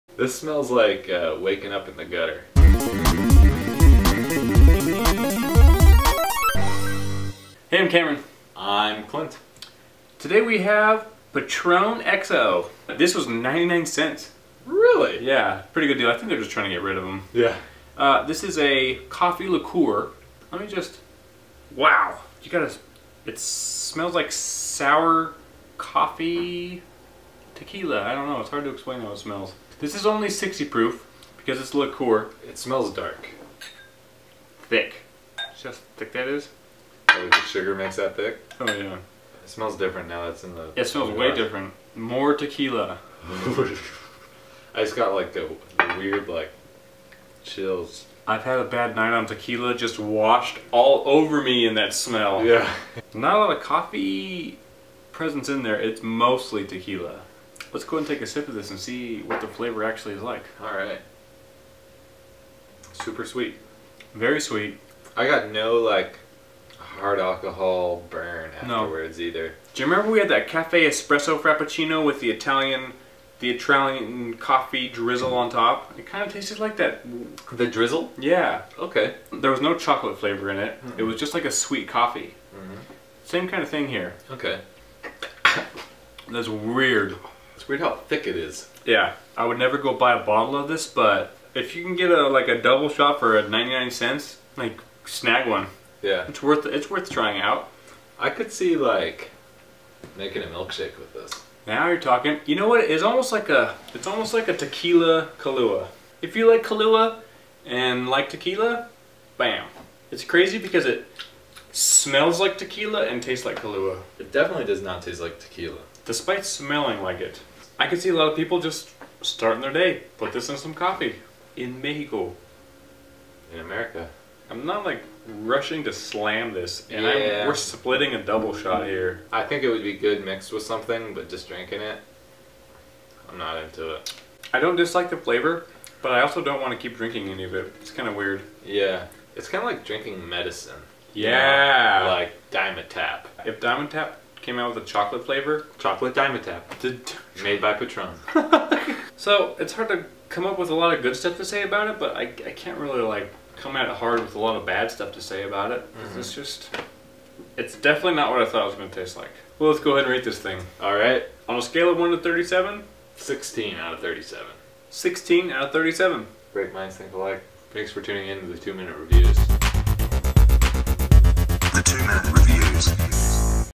Patron XO Cafe Liqueur - Extended Review - The Two Minute Reviews - Ep 320
The new coffee liquor available from Patron.